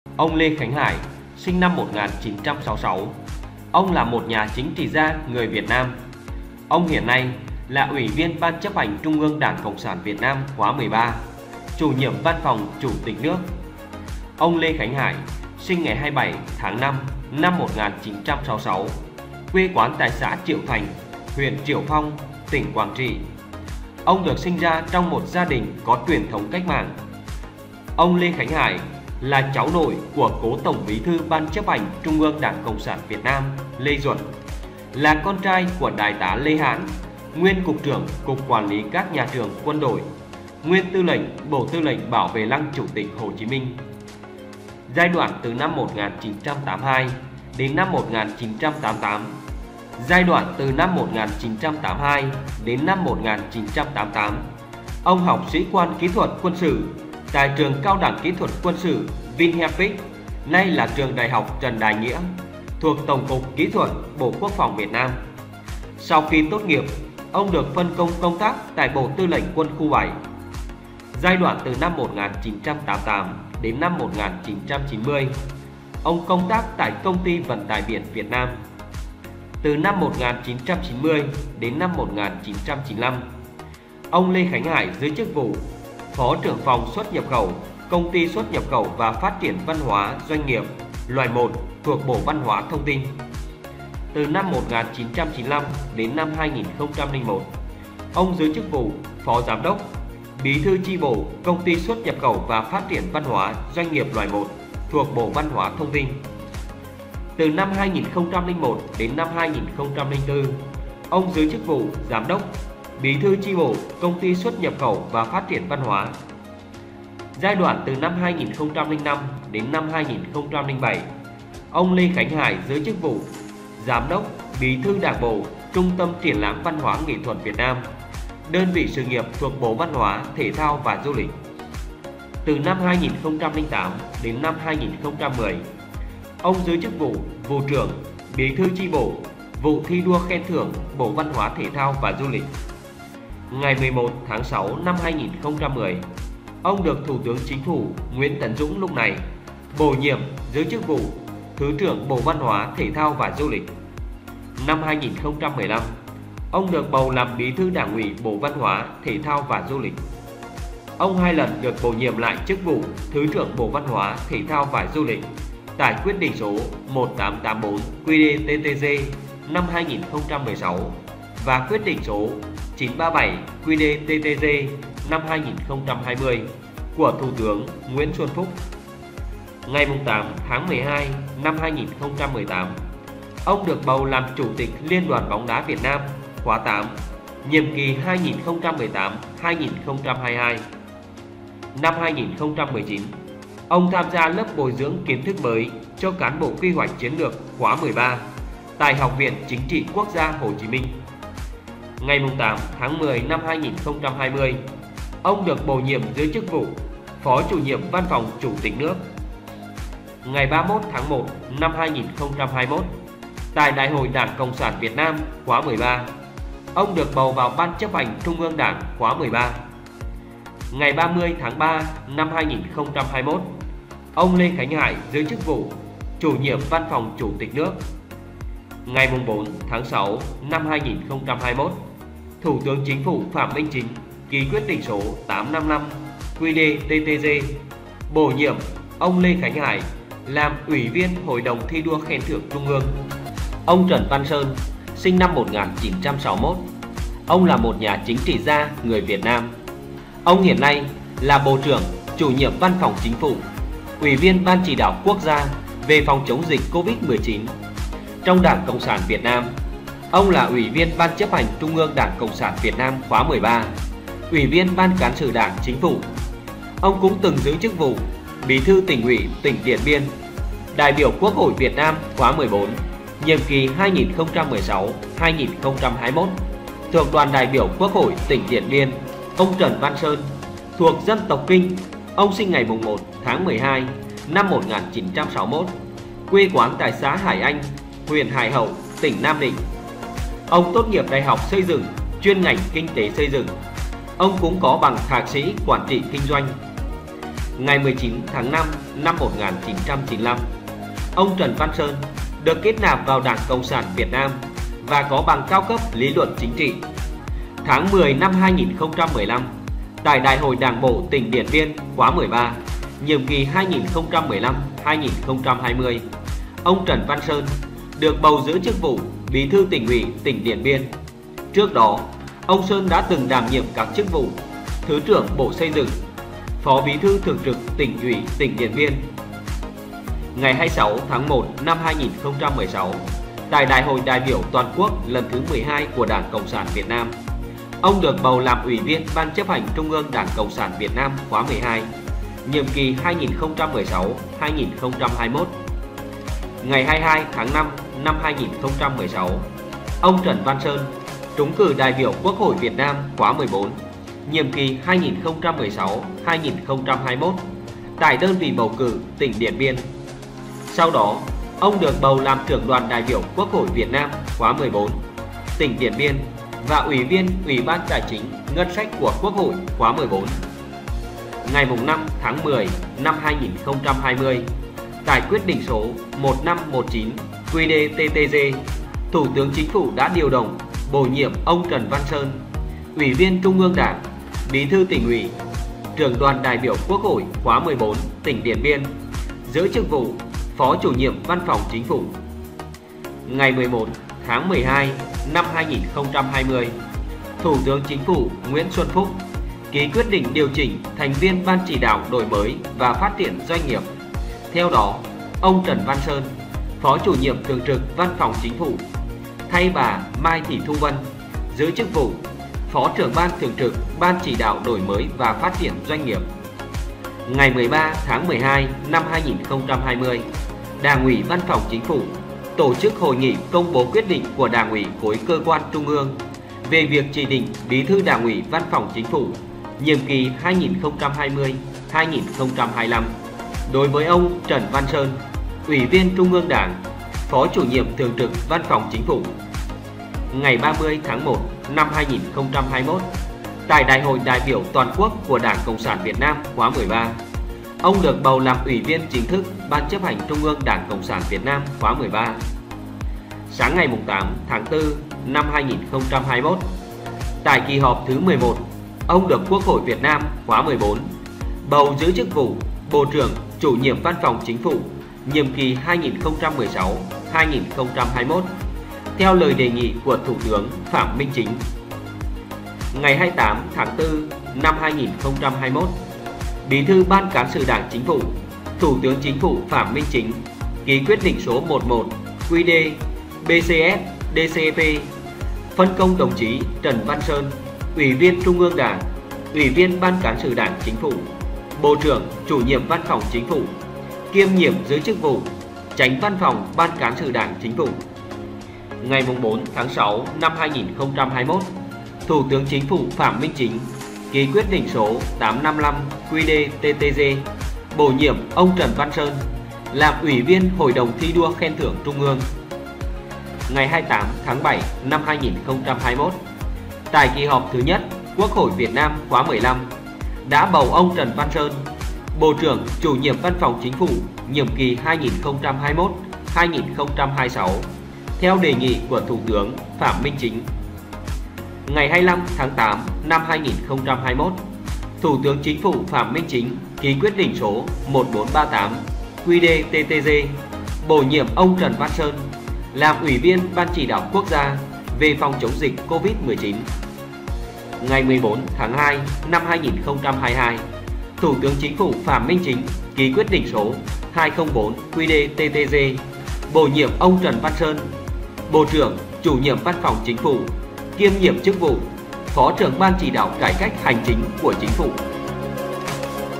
Tiểu sử 2 Chủ nhiệm của Văn phòng Chủ tịch nước và Văn phòng Chính phủ
Tiểu sử chủ nhiệm văn phòng Chủ tịch nước: Lê Khánh Hải (1966-) là một chính khách cao cấp Việt Nam và hiện là Ủy viên Ban Chấp hành Trung ương Đảng Cộng sản Việt Nam khóa XIII[1], Chủ nhiệm Văn phòng Chủ tịch nước.
Lê Khánh Hải sinh ngày 27 tháng 05 năm 1966, quê tại xã Triệu Thành, huyện Triệu Phong, tỉnh Quảng Trị,[3] trong một gia đình có truyền thống cách mạng, là cháu nội của cố Tổng Bí thư Ban Chấp hành Trung ương Đảng Cộng sản Việt Nam Lê Duẩn, là con trai của Đại tá Lê Hãn - nguyên Cục trưởng Cục Quản lý các nhà trường quân đội, nguyên Tư lệnh Bộ Tư lệnh Bảo vệ Lăng Chủ tịch Hồ Chí Minh.
Ngày 30 tháng 3 năm 2021, Ông Lê Khánh Hải giữ chức Chủ nhiệm Văn phòng Chủ tịch nước.
Chủ nhiệm văn phòng Chính phủ: Trần Văn Sơn (sinh năm 1961) là một chính trị gia người Việt Nam. Ông hiện là Bộ trưởng, Chủ nhiệm Văn phòng Chính phủ, Ủy viên Ban chỉ đạo Quốc gia phòng, chống dịch COVID-19. Trong Đảng Cộng sản Việt Nam, ông là Ủy viên Ban Chấp hành Trung ương Đảng Cộng sản Việt Nam khóa XIII, Ủy viên Ban Cán sự Đảng Chính phủ. Ông từng là Bí thư Tỉnh ủy Điện Biên, đại biểu Quốc hội Việt Nam khóa XIV nhiệm kì 2016–2021, thuộc đoàn đại biểu quốc hội tỉnh Điện Biên.
Ông Trần Văn Sơn, dân tộc Kinh, sinh ngày 1 tháng 12 năm 1961, tại xã Hải Anh, huyện Hải Hậu, tỉnh Nam Định.

Ông tốt nghiệp Đại học Xây dựng, ngành Kinh tế Xây dựng. Ông cũng có bằng Thạc sĩ Quản trị kinh doanh.
Sáng ngày 28 tháng 7 năm 2021, tại kỳ họp thứ 1 ông được Quốc hội Việt Nam khóa XV bầu làm Bộ trưởng, Chủ nhiệm Văn phòng Chính phủ nhiệm kỳ 2021–2026 theo đề nghị của Thủ tướng Phạm Minh Chính.
Người Nổi Tiếng là Blog cập nhật tiểu sử người nổi tiếng Việt Nam, thế giới.